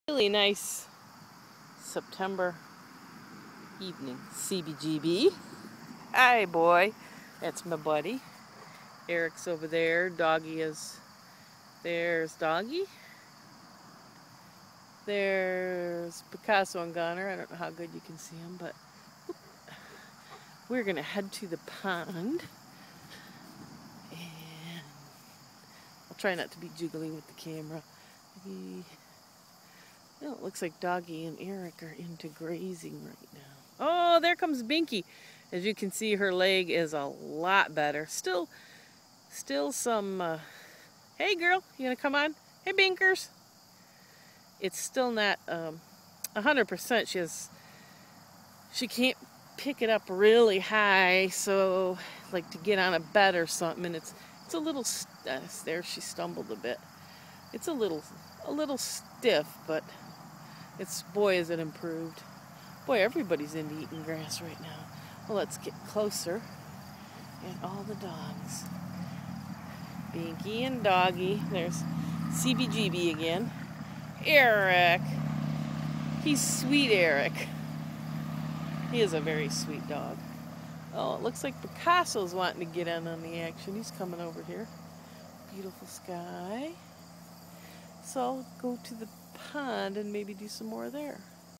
Pasture Sep 9, 2014
Wandering around with the dogs